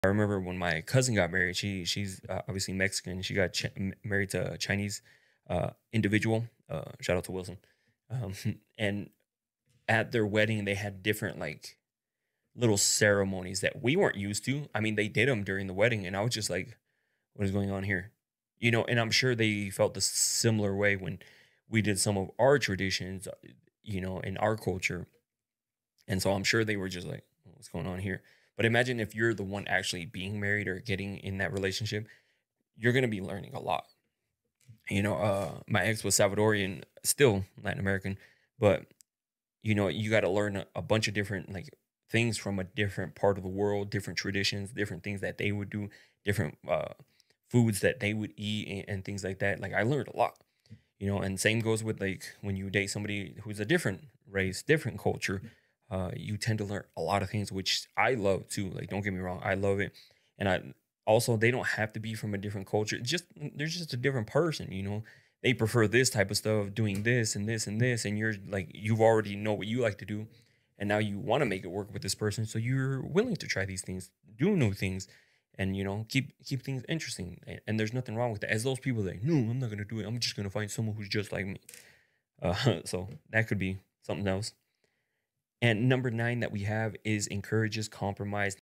Dating Someone from a Different Culture: Exploring Traditions and Embracing Differences
Discover the joy and challenges of dating someone from a different culture, as we share personal experiences and insights. Join us as we explore unique traditions, delicious foods, and the enriching journey of learning and growing together. Embrace diversity and create a stronger bond in your relationship.